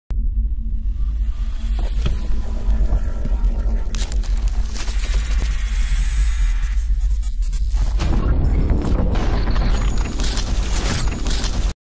Technic Spider Web Intro | After Effects
Been working on this the last few days, hope you enjoy.

This is an After Effects only project.